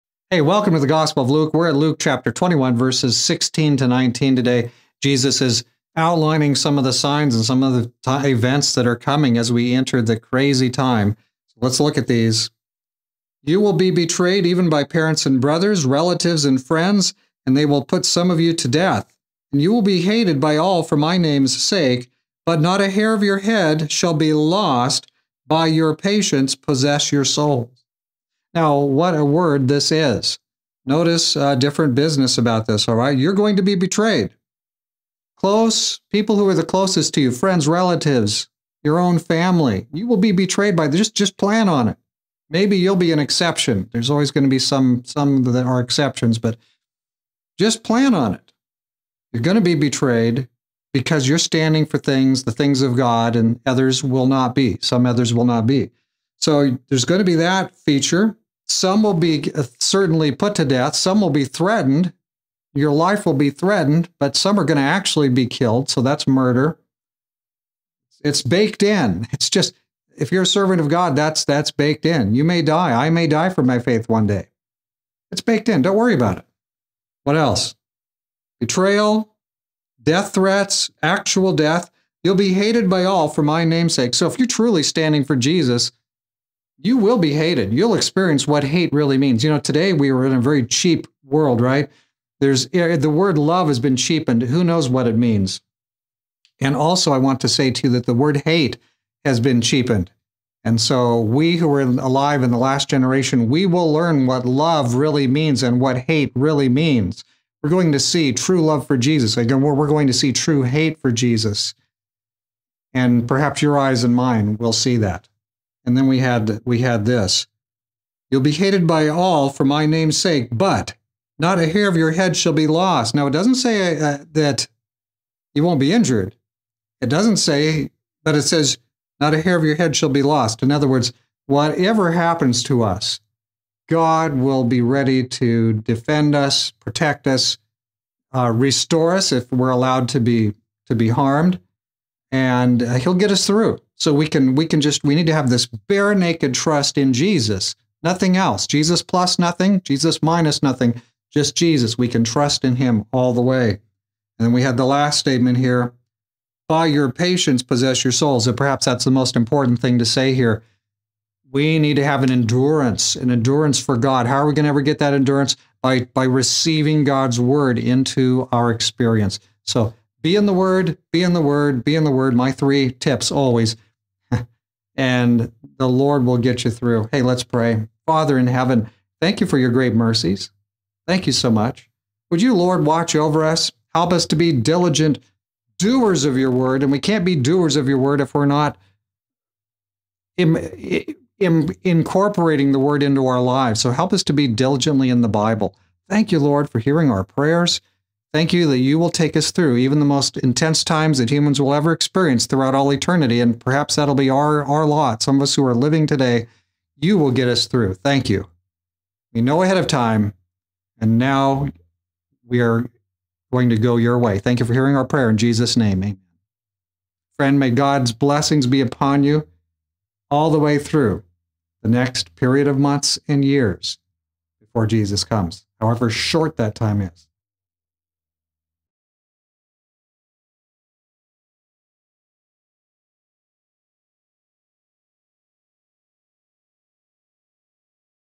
Luke 21:16-19 Faithful to the End
When we are true to Jesus how will that work out in the end? Will there be a price to pay? Will we even possibly be hated by most?